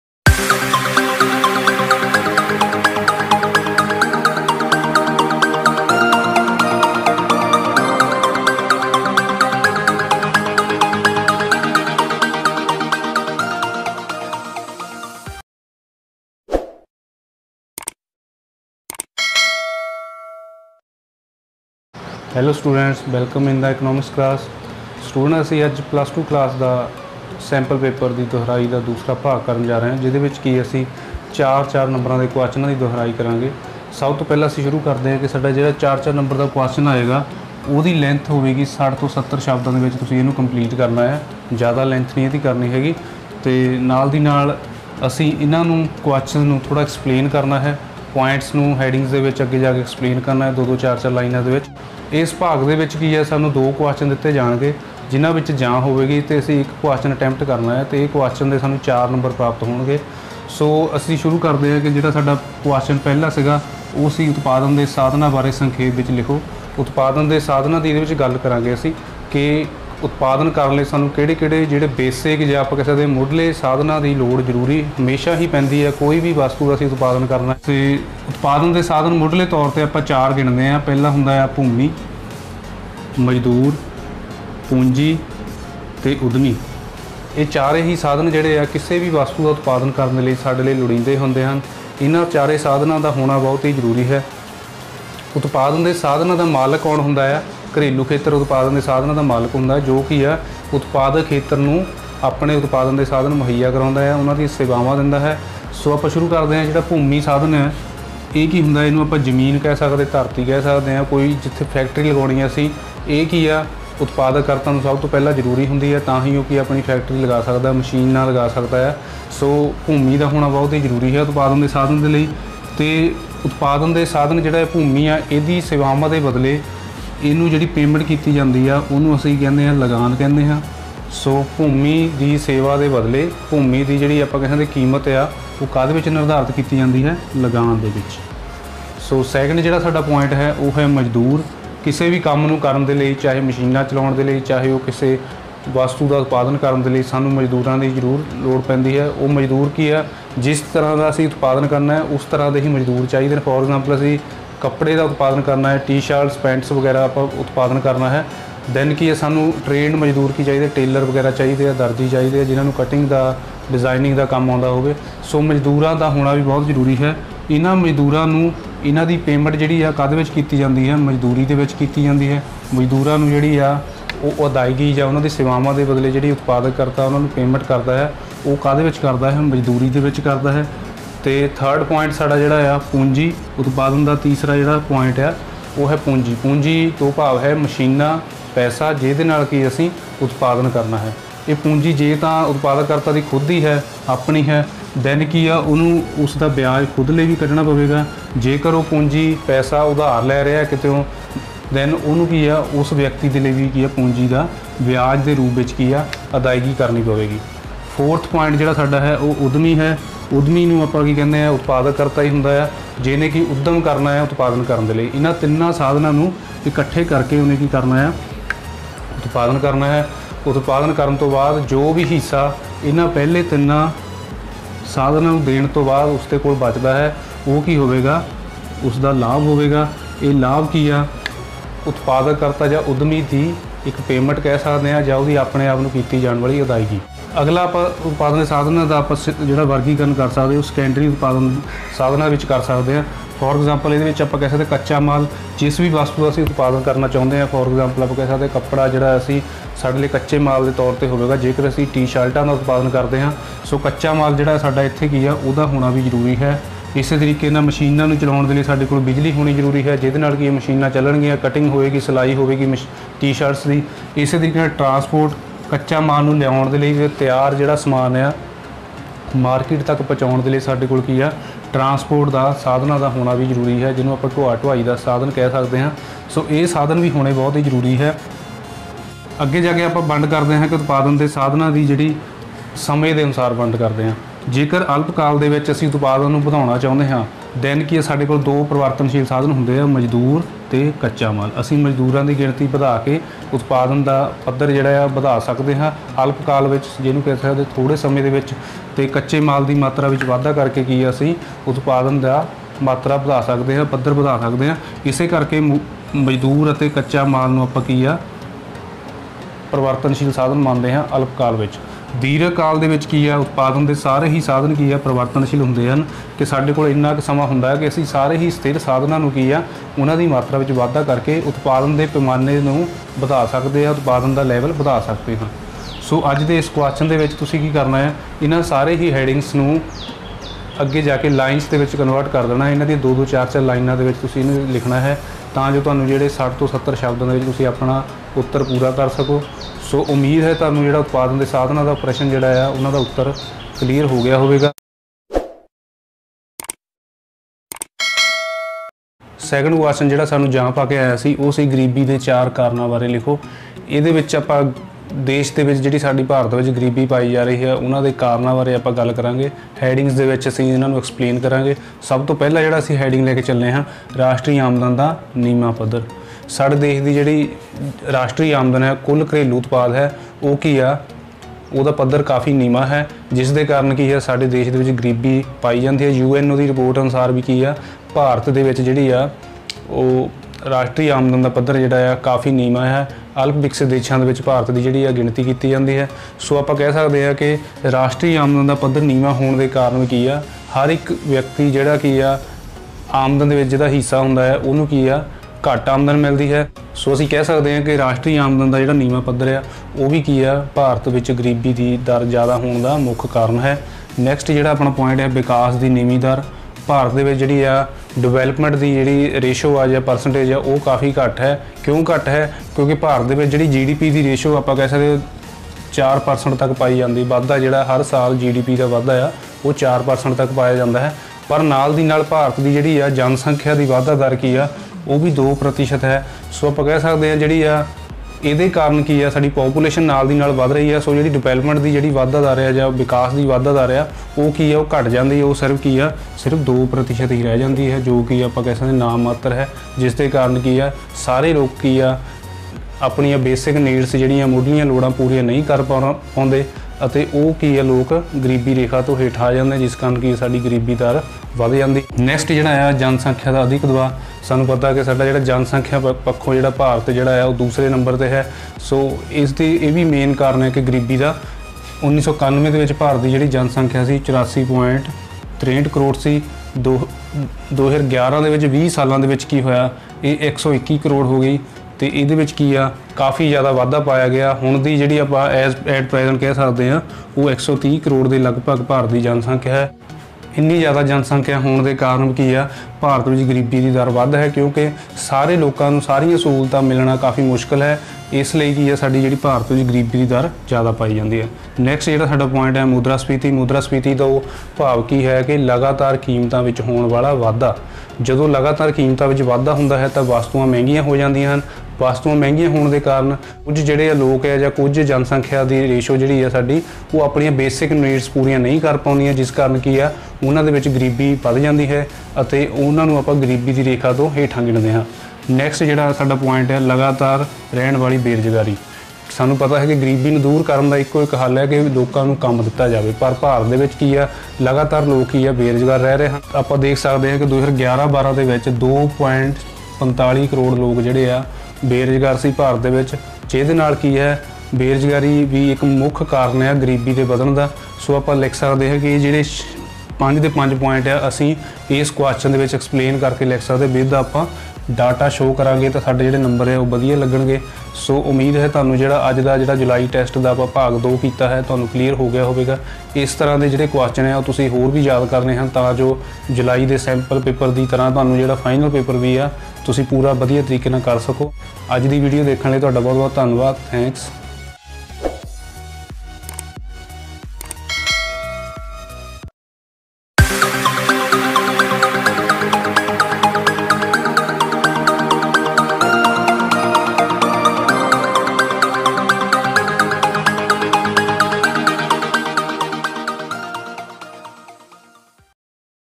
CLASS 12 ECONOMICS JULY SAMPLE TEST PAPER REVISION PART 2
SUBJECT: ECONOMICS TOPIC : JULY BIMONTHLY SAMPLE PAPER REVISION 4 MARKS QUESTIONS
PREPARED BY : R.K.BASRA
LECTURER ECONOMICS
GSSS MUSAPUR
S.B.S. NAGAR